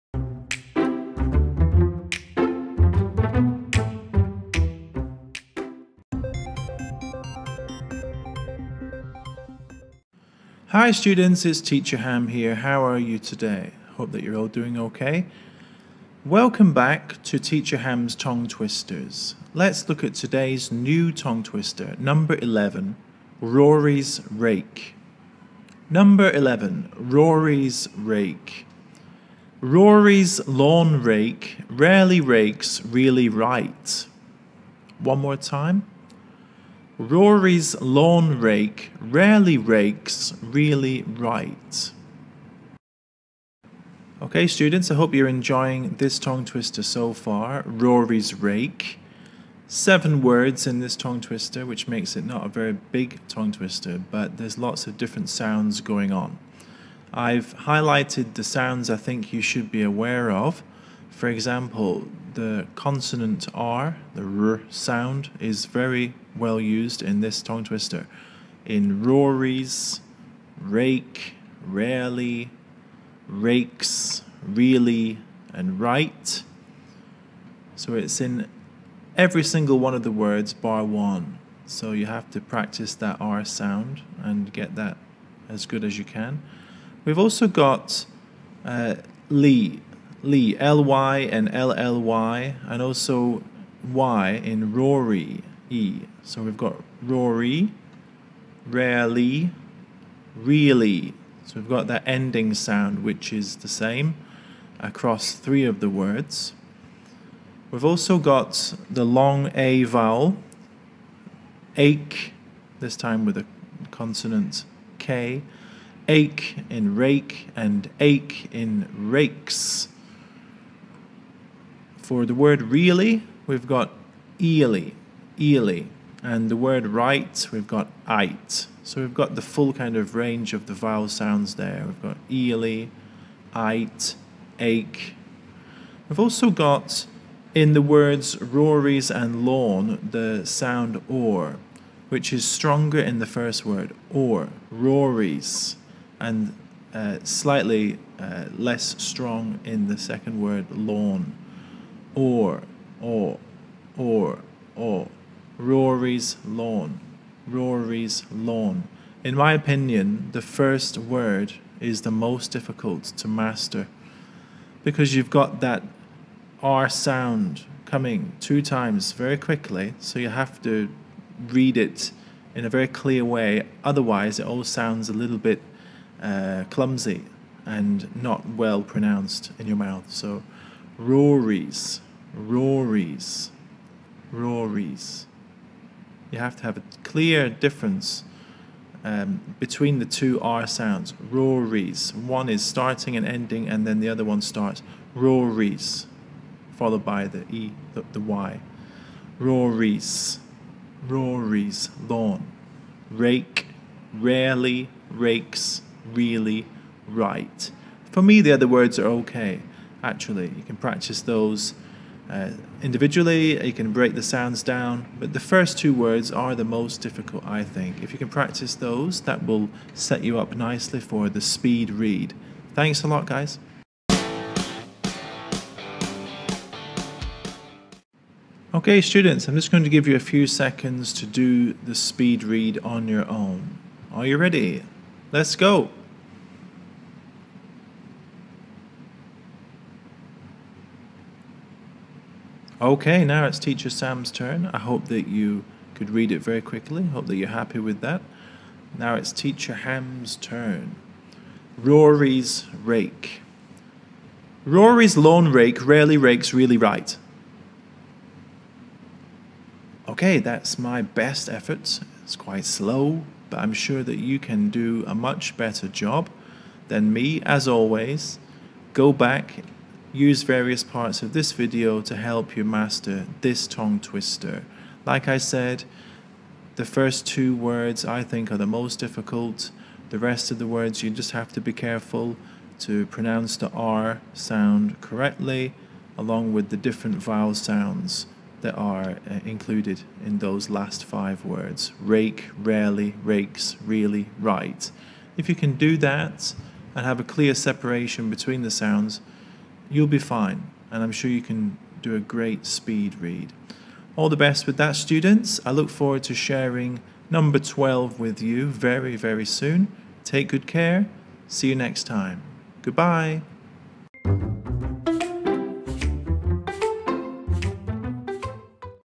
Teacher Ham's Tongue Twisters! No. 11
Teacher Ham's Tongue Twisters!
Truly terrifying tongue twisters for kids by Teacher Ham!

Have fun and improve your English speaking with this series of 25 tongue twisters.

No. 11 Rory's Rake

Rory’s lawn rake rarely rakes really right.

Sincere thanks to my good friend & fellow educator Mr Deta Clark for kindly sharing the tongue twisters.
Video Series developed by Samuel Alexander Barbour.